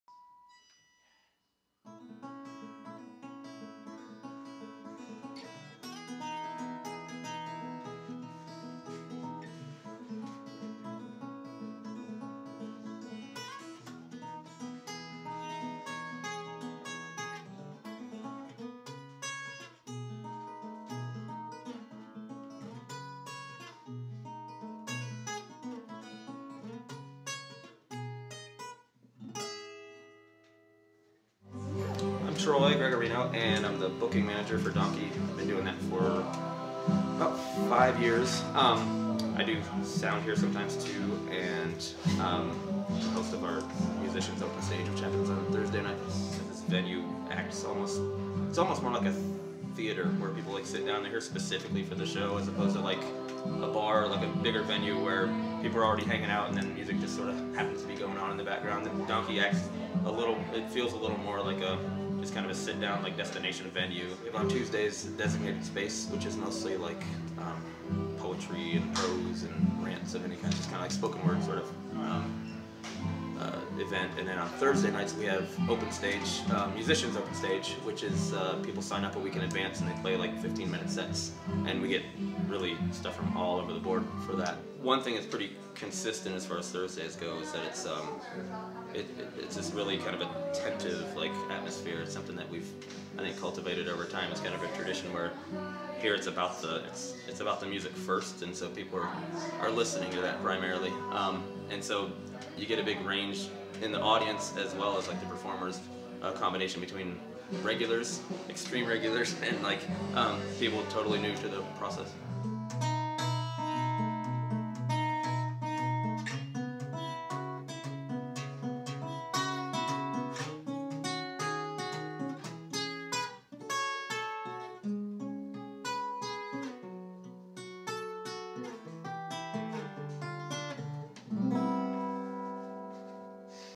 Donkey Coffee and Espresso: a "destination venue"
Donkey Coffee and Espresso is a local coffee shop located in the college town Athens, Ohio. This video looks at Donkey as a venue through an interview with Donkey's booking manager and sound guy, Troy Gregarino. The video also features various artists that performed at Donkey March 28th and 29th.